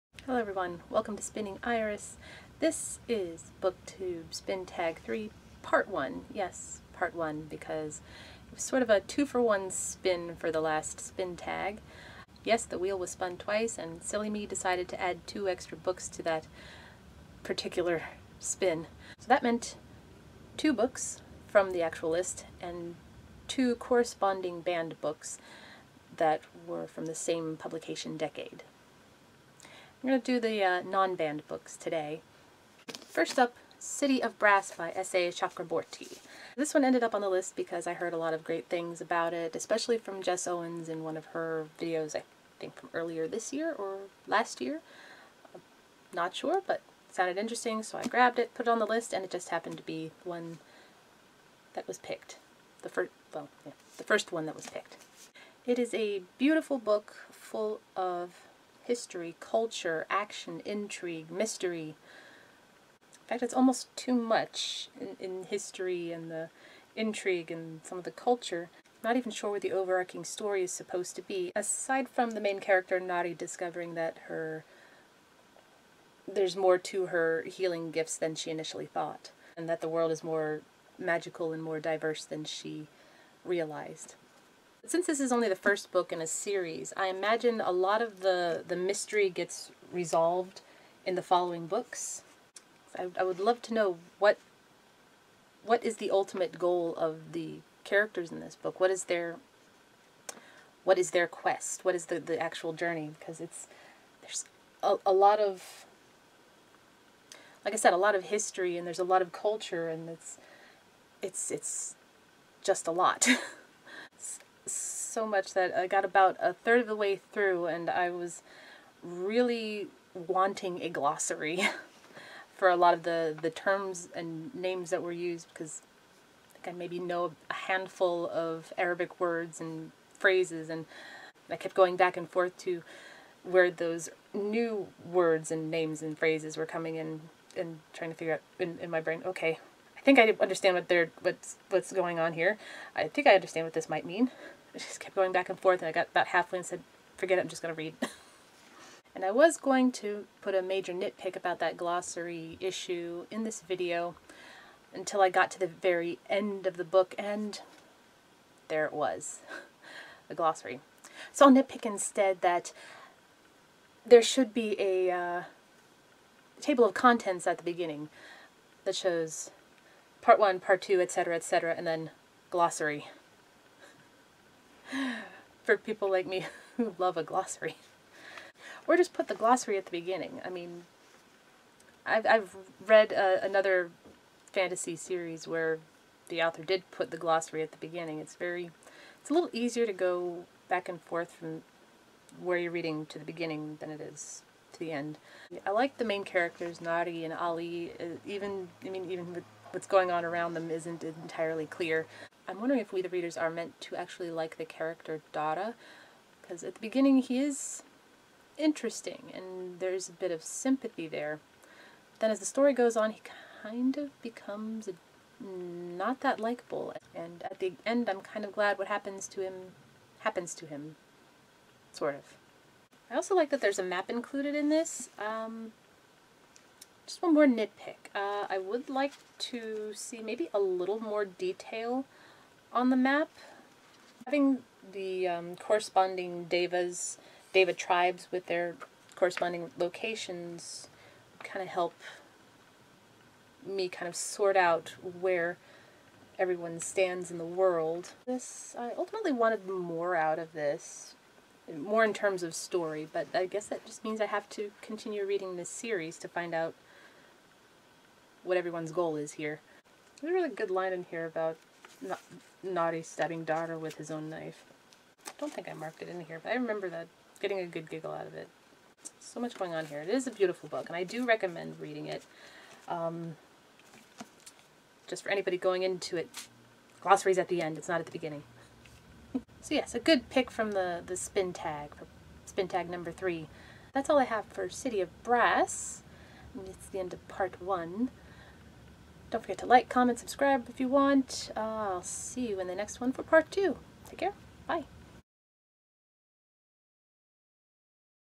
City of Brass: BookTube Spin 3 Part 1
First out of Four for the BookTube Spin Tag #3! Just some quick thoughts for City of Brass. Spoiler Free.

City of Brass, by S.A. Chakraborty